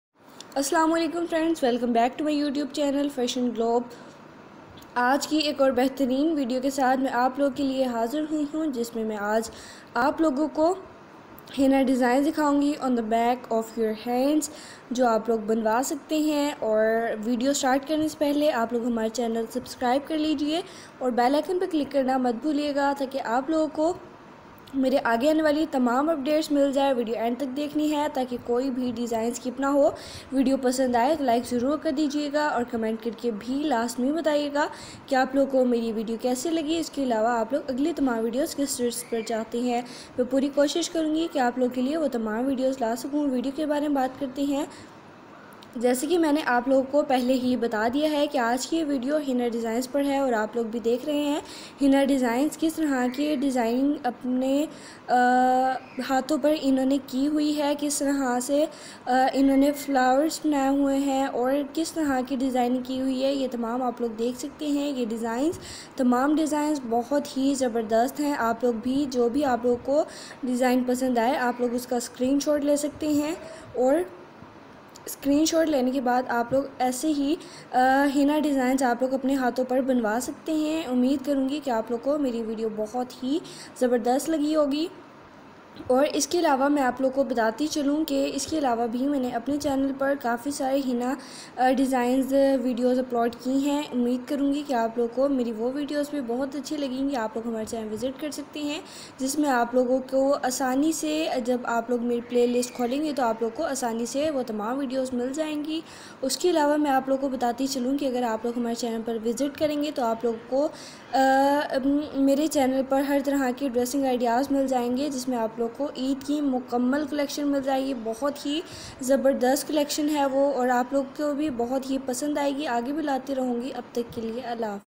Top 50 Mehandi Design Collection For Backhand For Eid #2021//Henna Mehandi Designs for back of hands
Hope so u liked it.